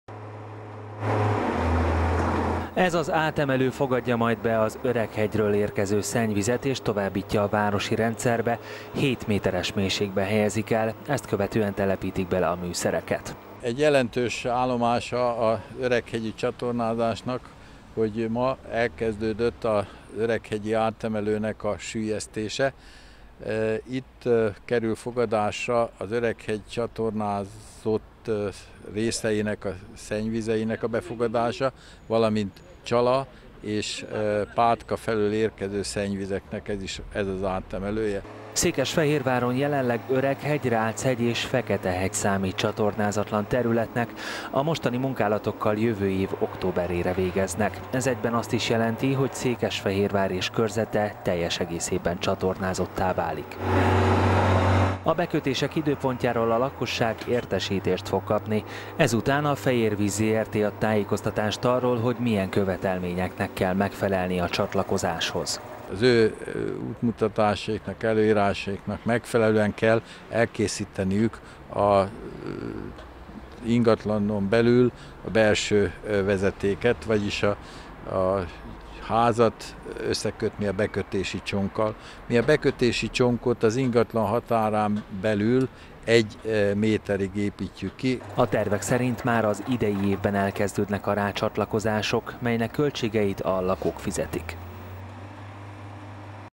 Építik az átemelőt a Fiskálison
Elkezdődött az öreghegyi szennyvíz átemelő süllyesztése. Ez a szerkezet emeli majd át a városi rendszerbe az ide érkező szennyvíz mennyiséget. A munkálatok várhatóan jövő év végére készülnek el, de a lakosság a tervek szerint már korábban rácsatlakozhat a kiépülő csatornarendszerre.